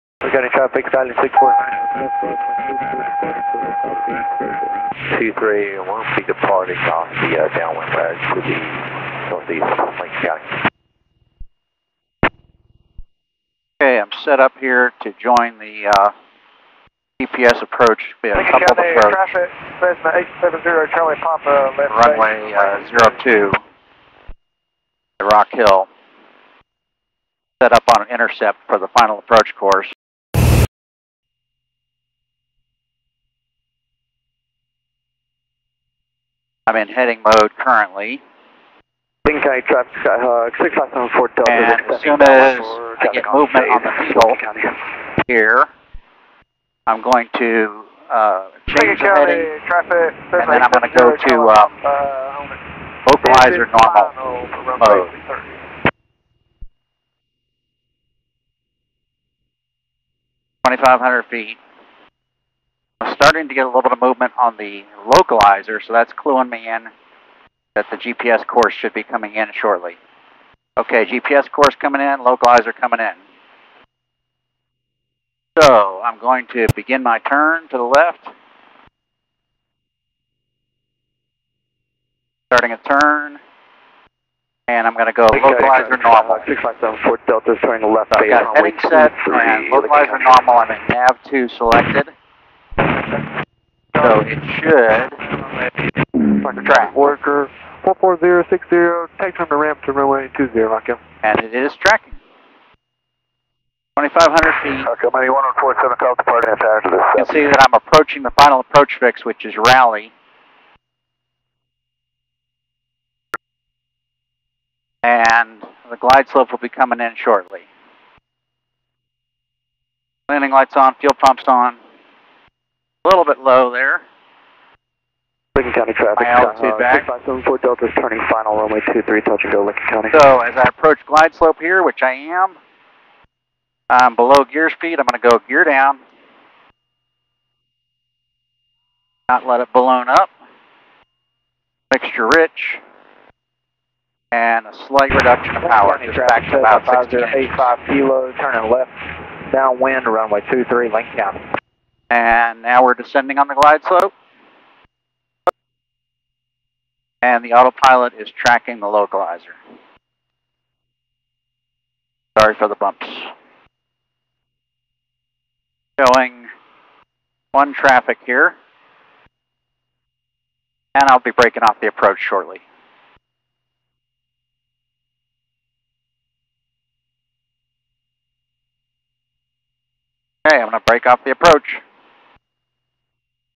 Flying a Piper Comanche 250 GPS coupled approach intercept with Century II autopilot
Using modern avionics with a middle age autopilot in a senior citizen airplane. Flying the RNAV LPV approach to runway 02 at KUZA, Rock Hill SC.